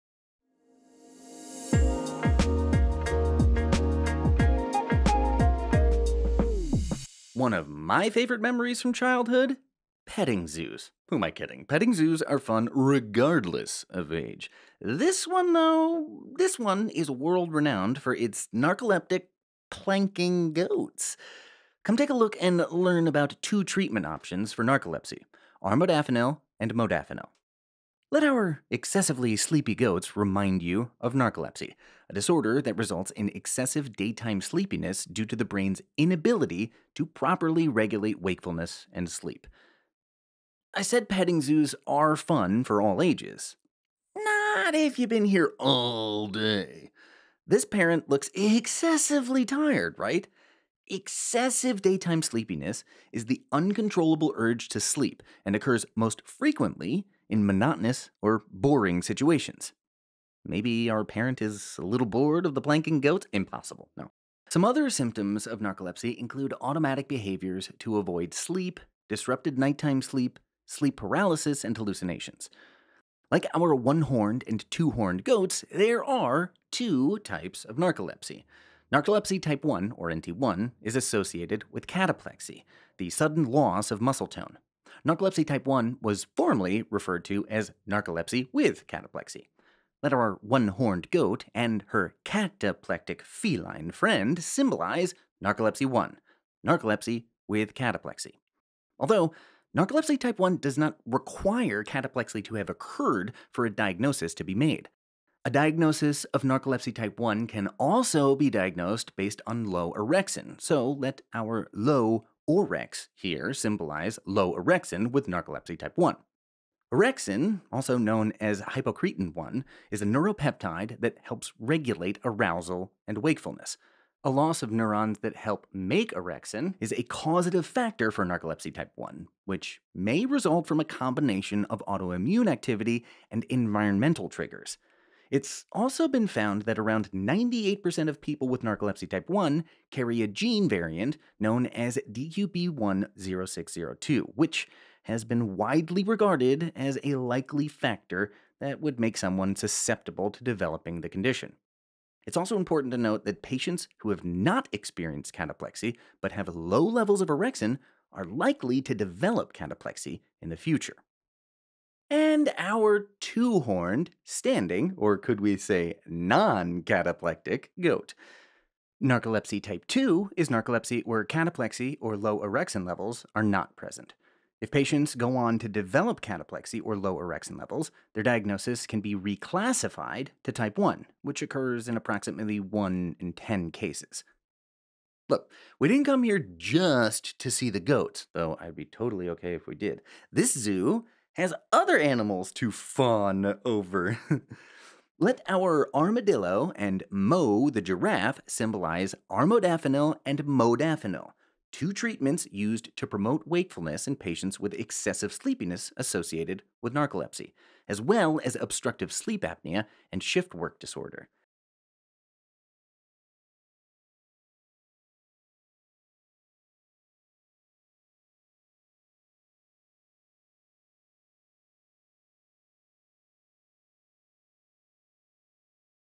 Narcolepsy Types & Treatment: Armodafinil & Modafinil (Part 1) | Sketchy Medical | USMLE Step 1
Ready to tackle your clerkships?

 This lesson introduces two treatment options for narcolepsy: armodafinil and modafinil. It explains the symptoms and types of narcolepsy, along with the role of orexin and genetic factors. The video also highlights the potential side effects and precautions of these medications, including their effects on other drugs and pregnancy. Enjoy this preview? Get the rest of this lesson and see hundreds more on sketchy.com.